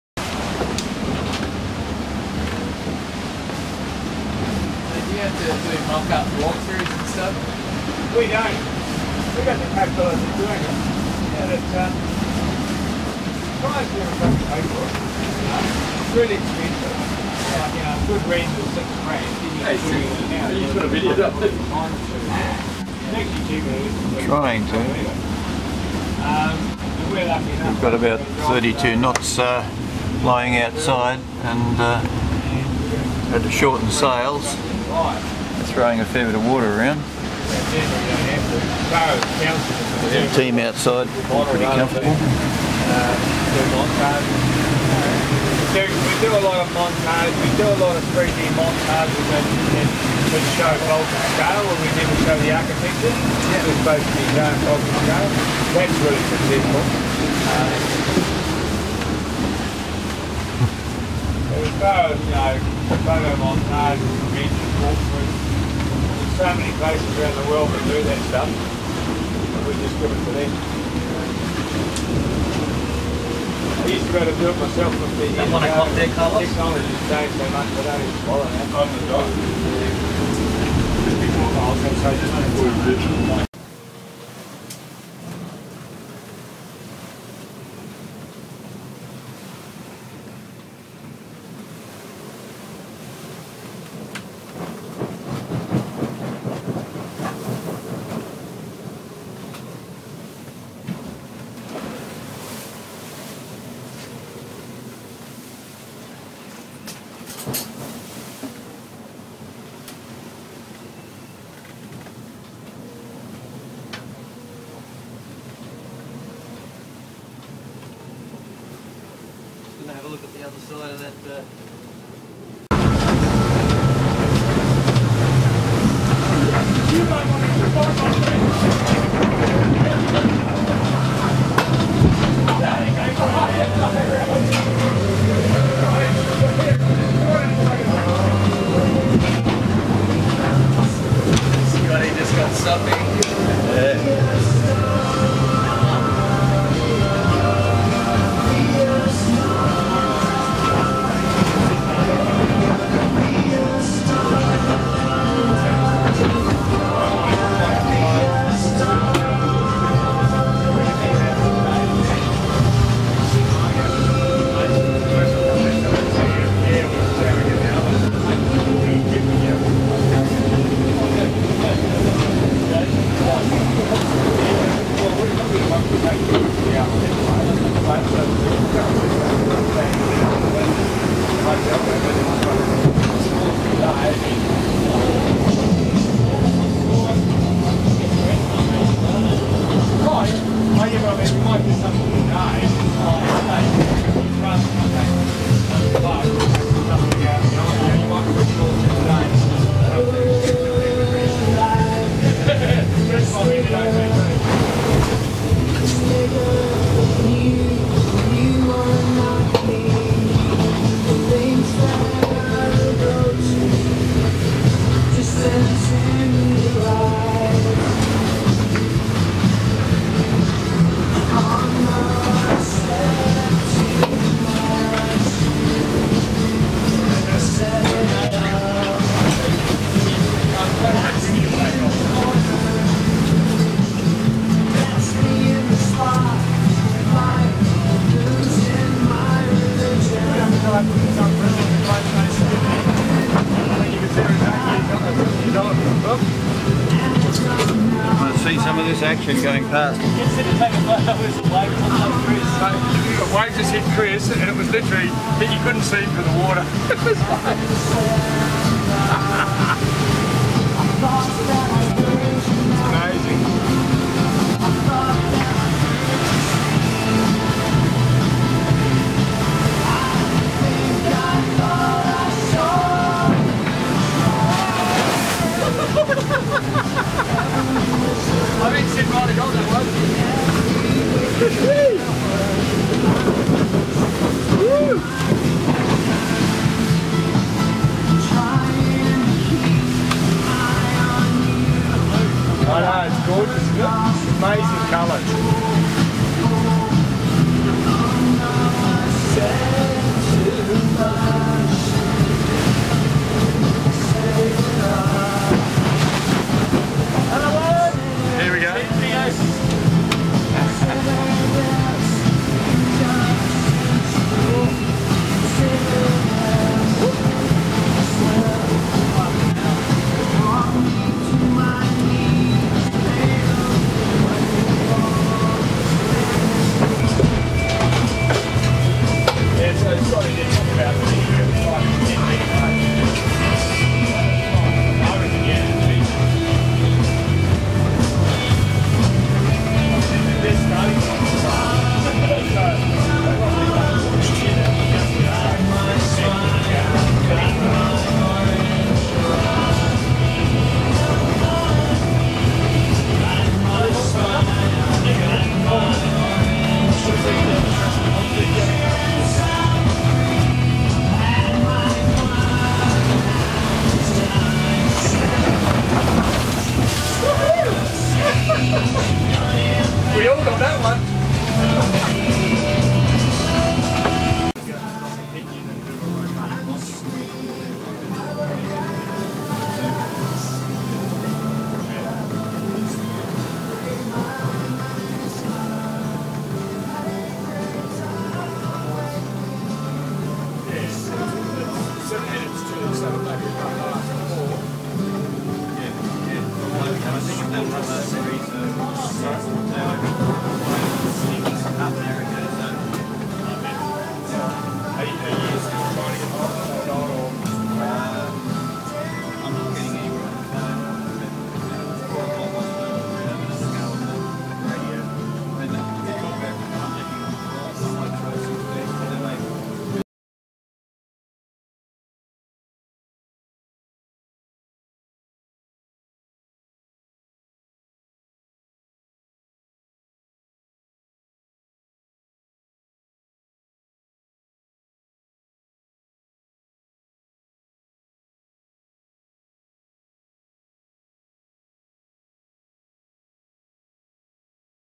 2015 Lord Howe Trip 32 to 60 plus knots
Star of Antlia, a Buizen 48, travelling from Sydney to Lord Howe Island at Easter 2105. A low formed near Lord Howe giving us winds to 60 knots and building seas. Video clips are of the boat and crew in 30-50 knot conditions.